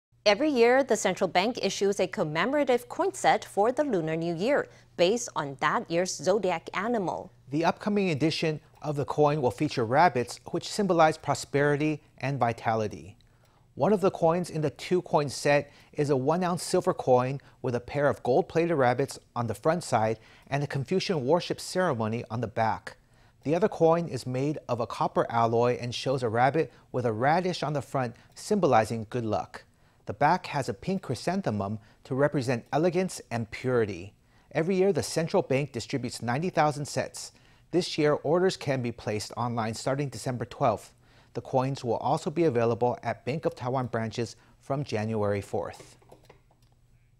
Preorders for Year of the Rabbit commemorative coin set to open online on Dec. 12
Every year, the central bank issues a commemorative coin set for the Lunar New Year, based on that year’s zodiac animal. The upcoming edition of the coins will feature rabbits, which symbolize prosperity and vitality.

One of the coins in the two-coin set is a 1-ounce silver coin with a pair of gold-plated rabbits on the front side and a Confucian worship ceremony on the back. The other coin is made of a copper alloy, and shows a rabbit with a radish on the front, symbolizing good luck. The back has a pink chrysanthemum to represent elegance and purity. Every year the central bank distributes 90,000 sets. This year, orders can be placed online starting Dec. 12. The coins will also be available at Bank of Taiwan branches from Jan. 4.